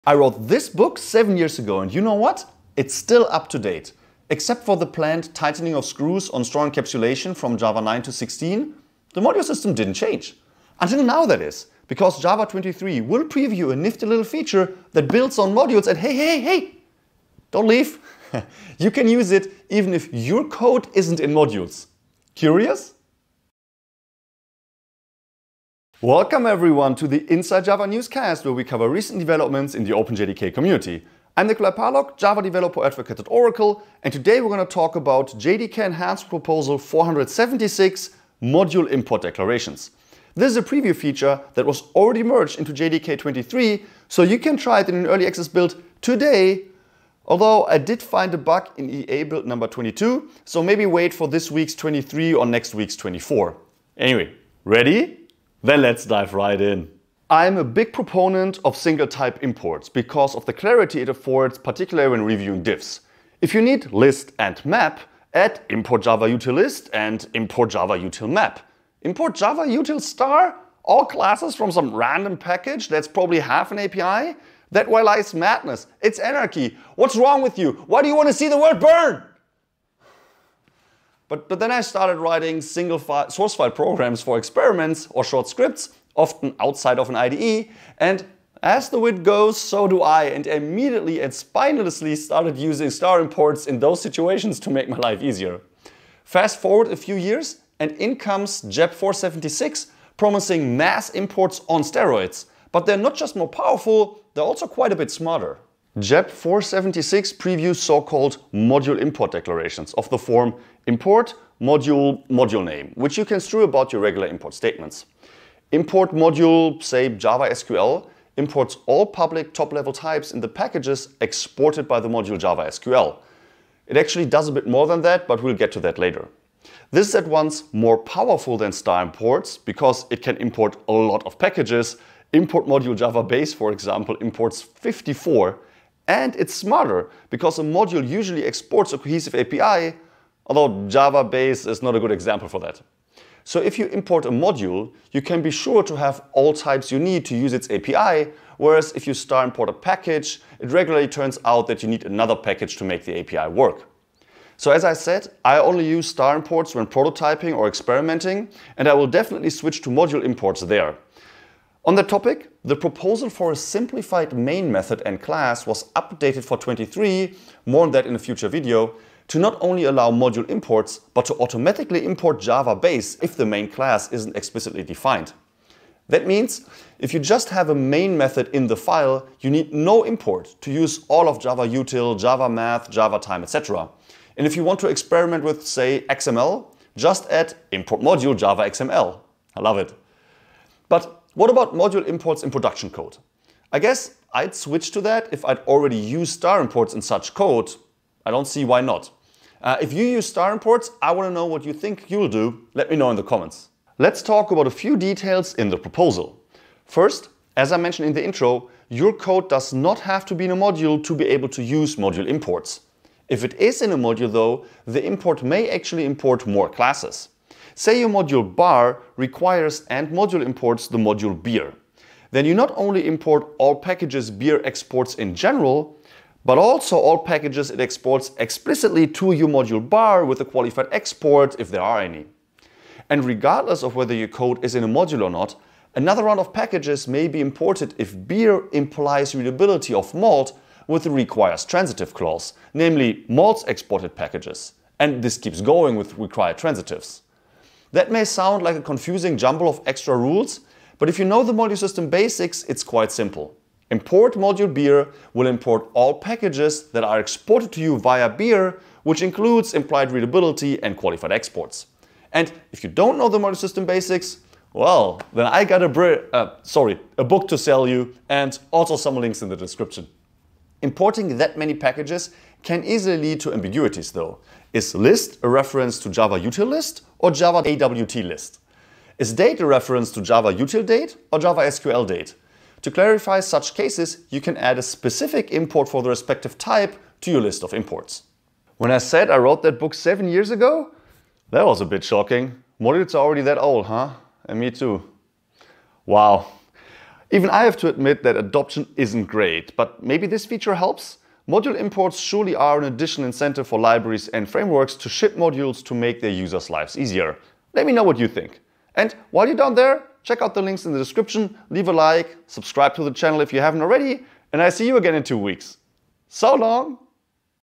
Module Imports in Java 23 - Inside Java Newscast #69
To reduce the overhead of using APIs, particularly in single source files, Java 23 previews module import declarations of the form `import module $moduleName, which import all packages exported by the named module.

~~~ Chapters ~~~

0:00 Intro
0:57 Star Imports
1:46 Module Imports
3:24 Module Import Details
4:48 More Modules?